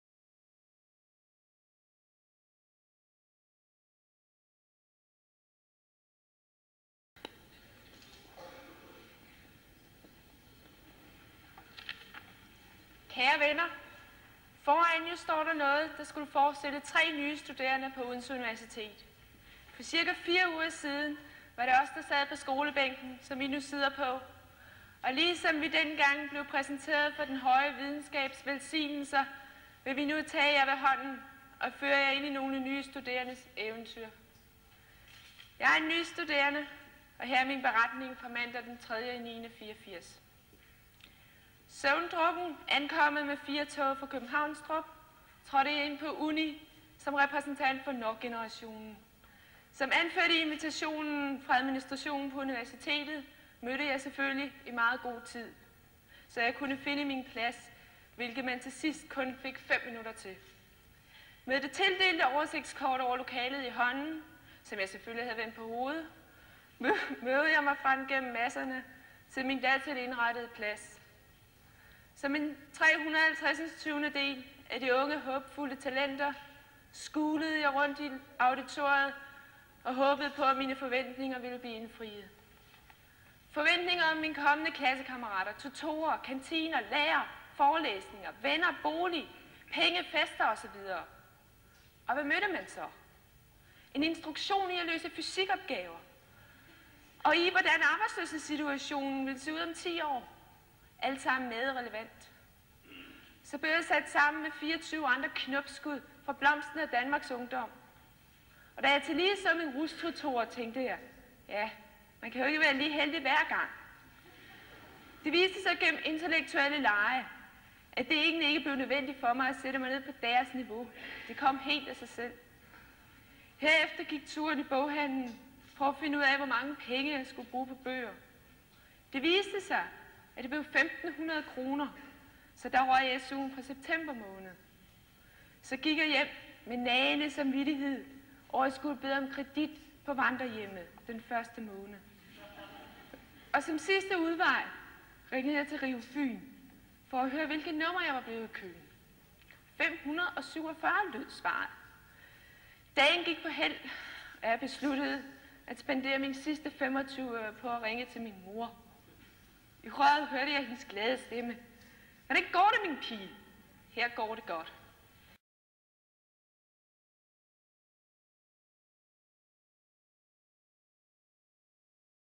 Film fra Årsfest 1984 - Studentertale 1
Første tale fra studerende ved Odense Universitet til Årsfest 1984. Dronning Ingrid var årets royale gæst. Hun sidder umiddelbart foran talerstolen. Som det kan høres i talerne, var de studerende ikke inviteret med til årsfesten.